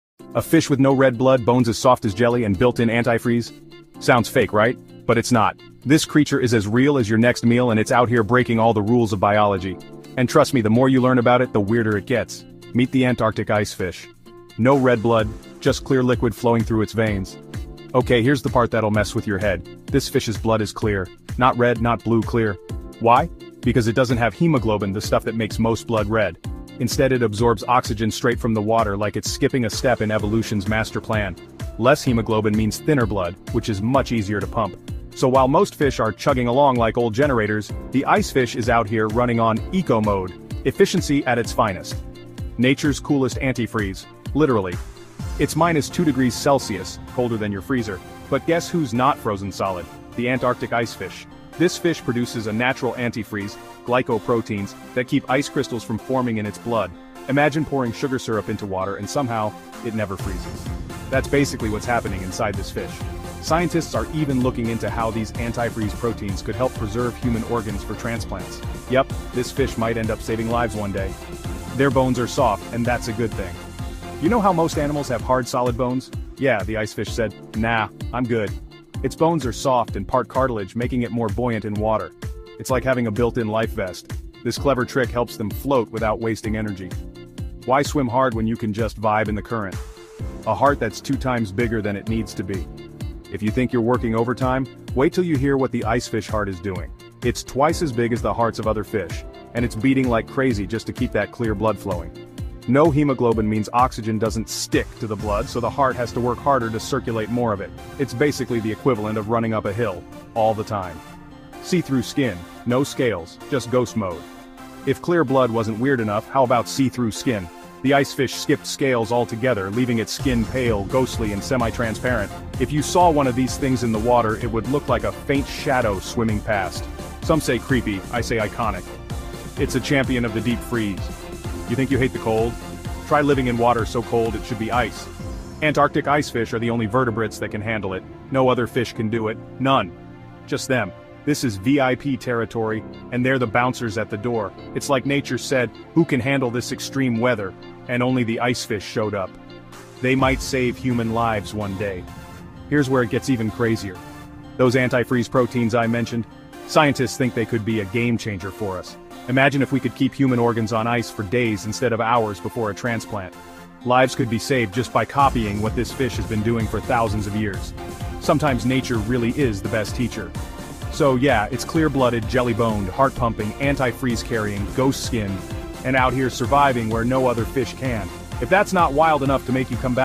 This Fish Has No Red Blood and Survives -2°C Waters
The Antarctic Icefish